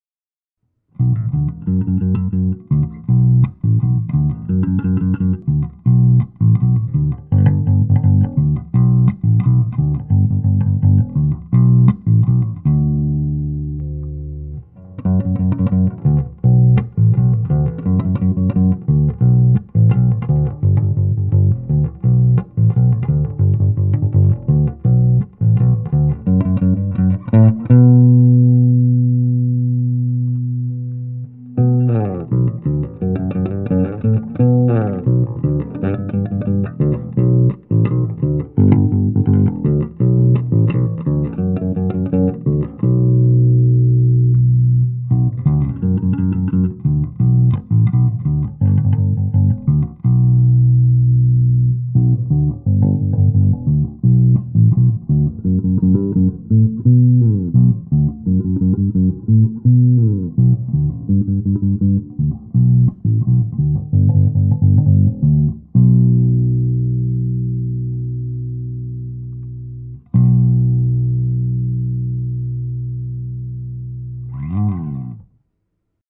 GUITARE COLLECTION presents Fender Jazz Bass JV from 1983 by Jérémie Francblu
Jérémie is playing a 1983 Fender Jazz Bass JV Medium Scale in our first shop in Paris !